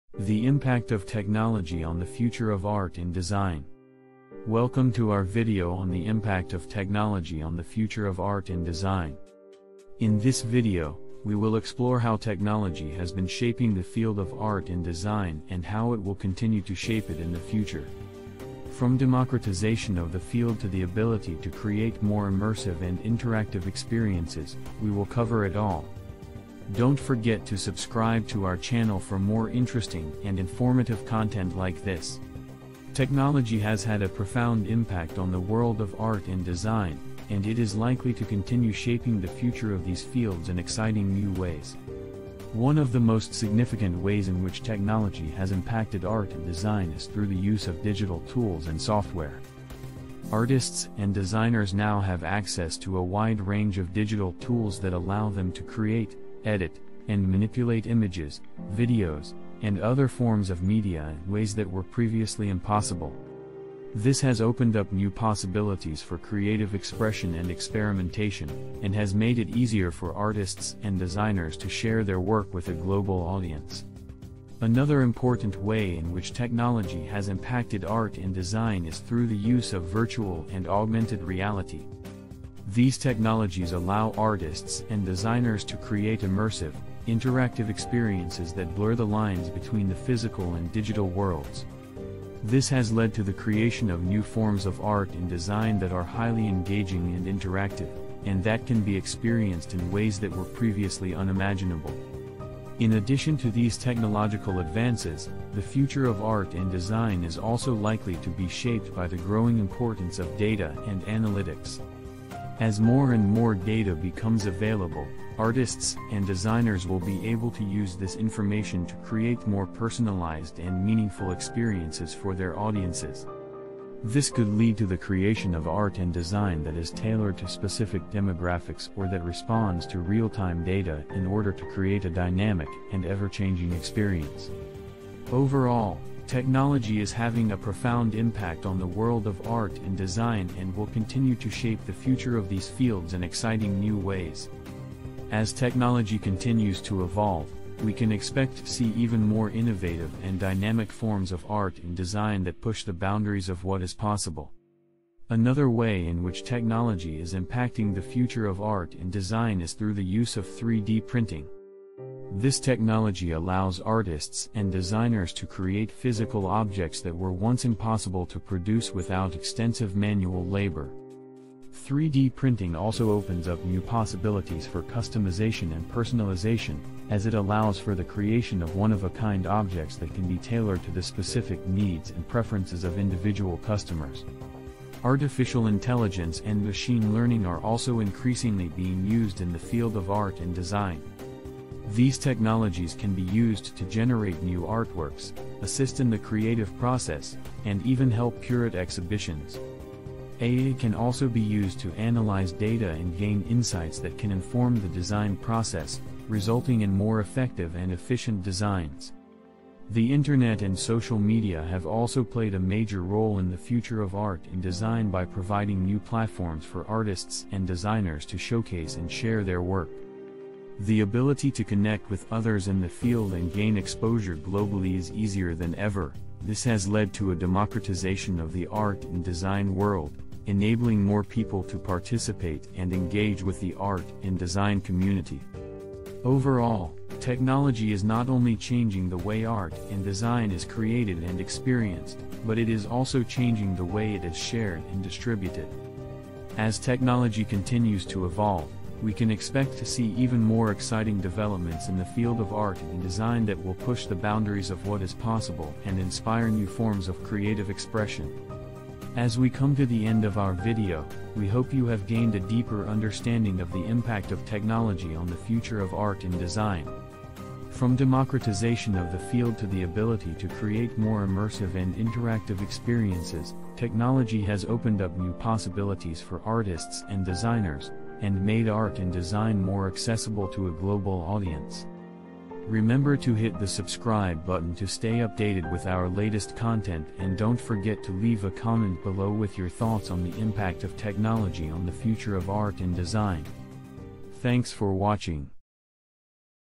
The impact of technology on the future of art and design
Join us as we explore the impact of technology on the future of art and design. From democratization of the field to the ability to create more immersive and interactive experiences, we delve into how technology is shaping the field of art and design and how it will continue to shape it in the future. This is a must-watch for art and design enthusiasts or anyone interested in understanding the impact of technology on the future of these fields.